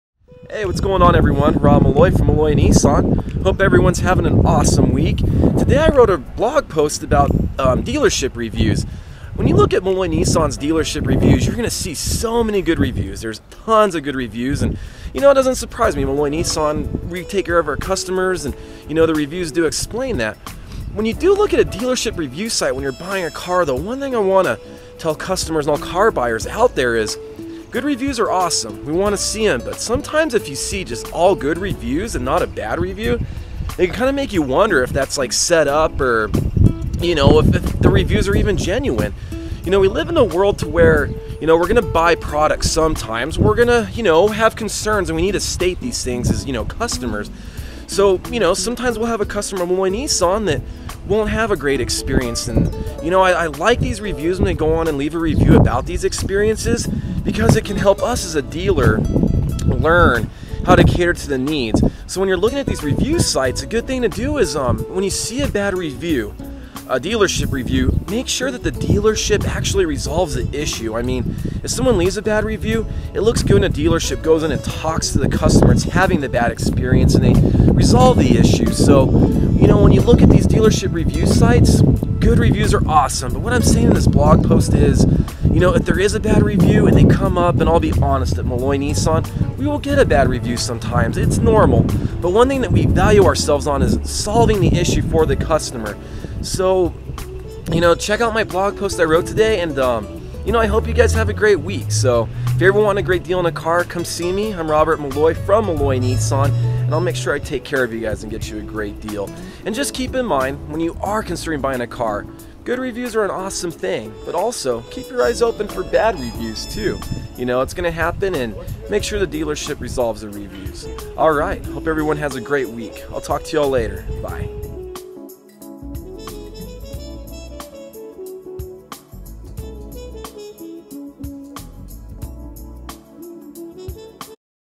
Bad Dealership Reviews Help Dealerships Cater To Customer Needs.
This video can be found on our blog In this video I share my thoughts on good and bad dealership reviews and how these reviews can educate customers before they purchase a car!